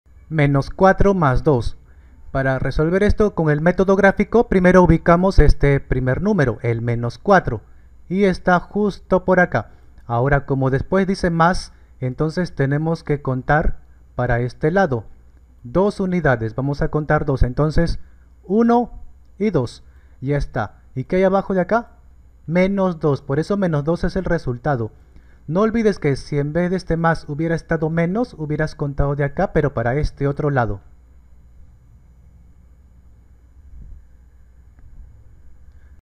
menos 4 mas 2 , suma y resta de numeros enteros metodo grafico -4+2=
suma y resta de numeros enteros metodo grafico
como aplicar el metodo grafico para operaciones con numeros enteros